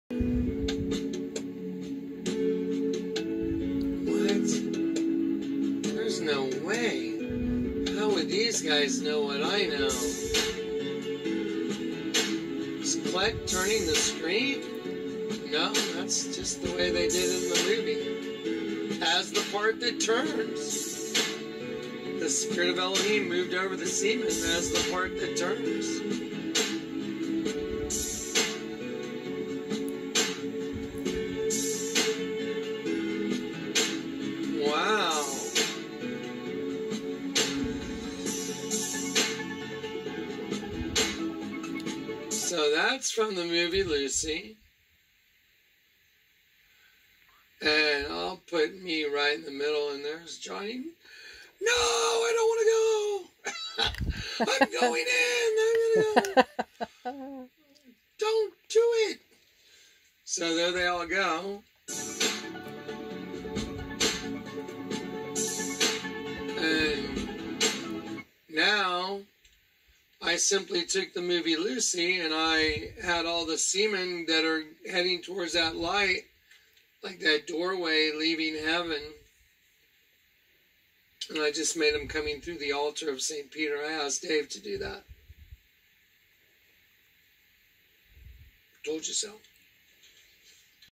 Feminine Energy taking over! Don't be deceived! Your birthed into a prison for your insurrection.
The Holy SPIRIT is not female, not a mother!

Robert! Carpenter! And ZEN...please seek the LORD God, I know he will put you right!